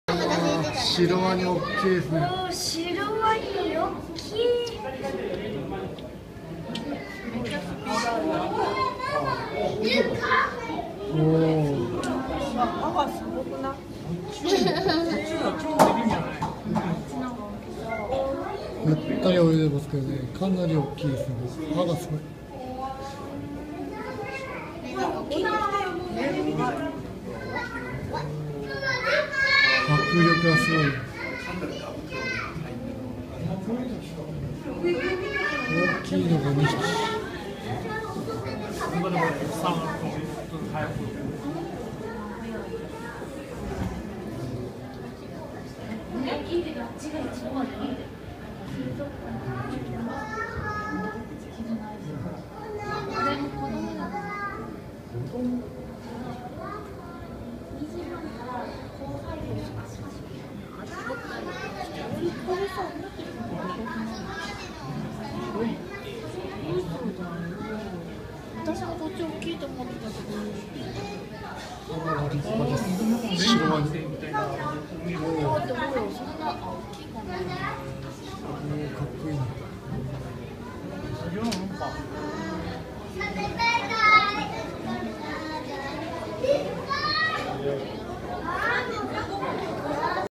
巨大ザメ！品川水族館のシロワニ
品川水族館の見所のひとつ。シャーク水槽。
巨大なシロワニが悠然と泳ぐ姿は迫力満点！